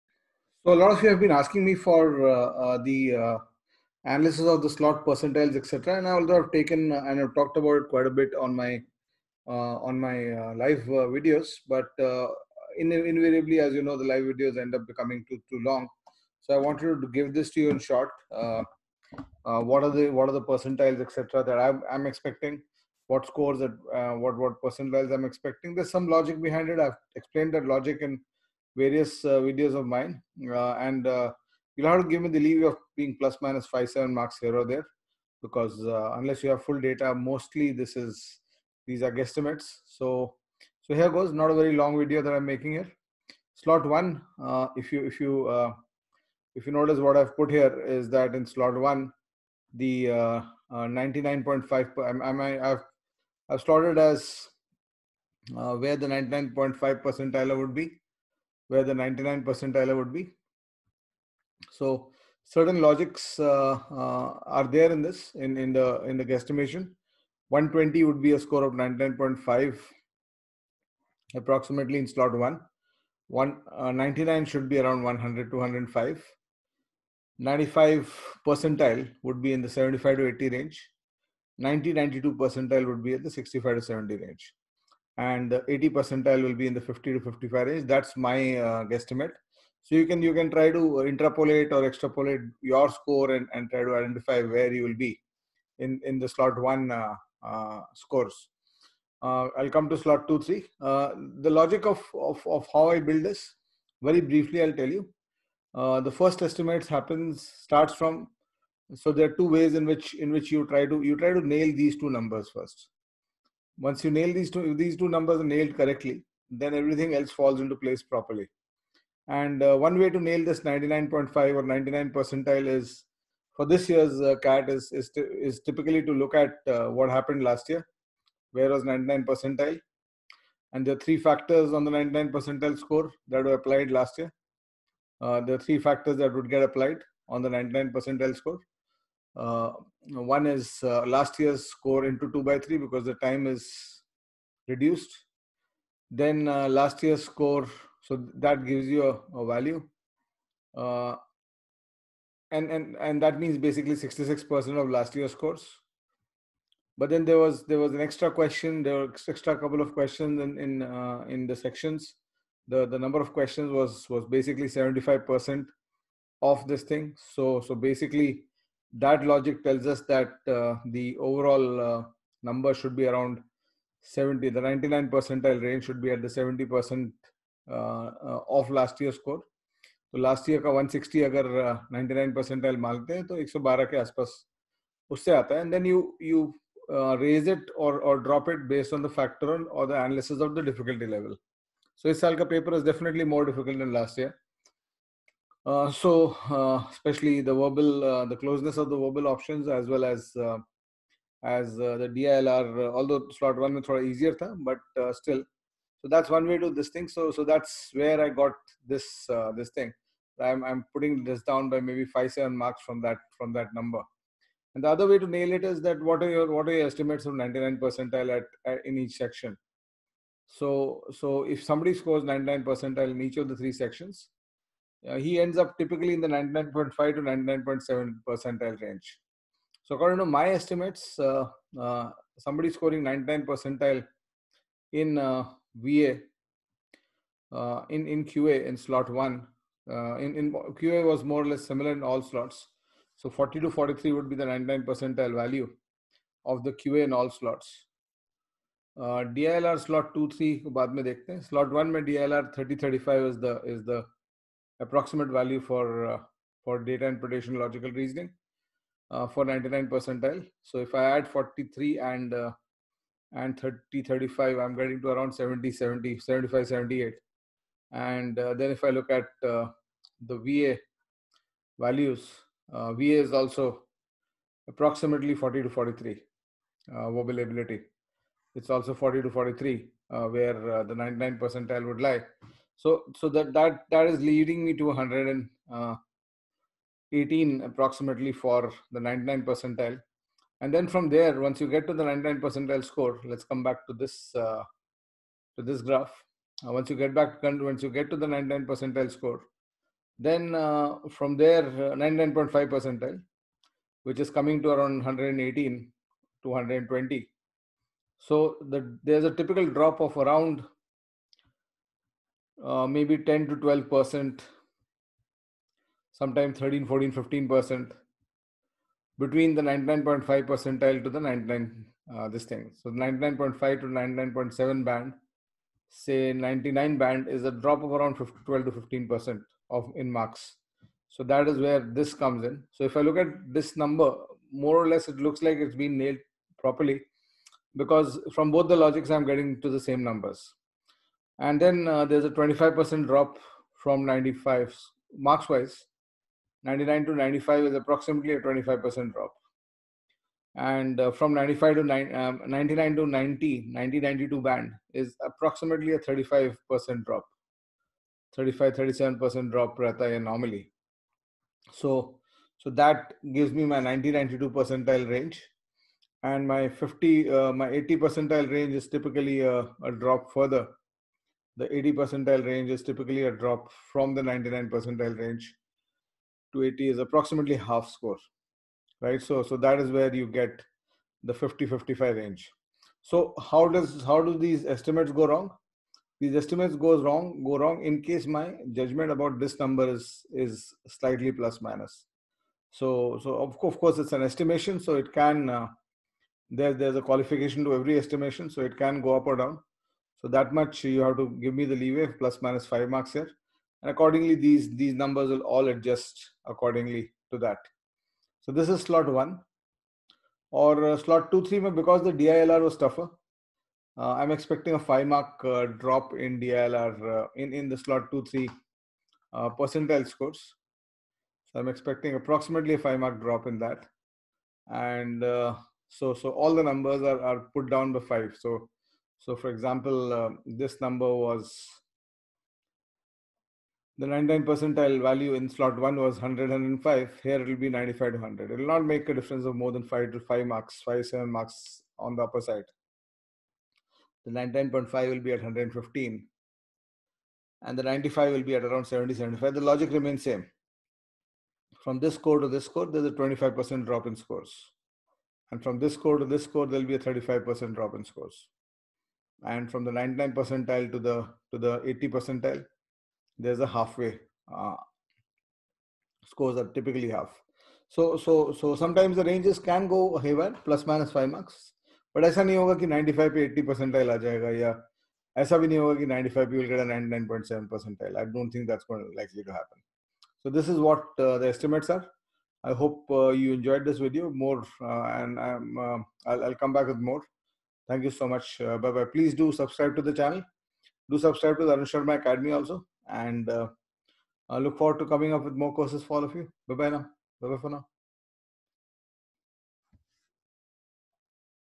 CAT 2020 SCORE VERSUS PERCENTILE PREDICTION
In this video, Arun Sharma discusses the possibilities of the percentiles at different scores in the CAT 2020 paper.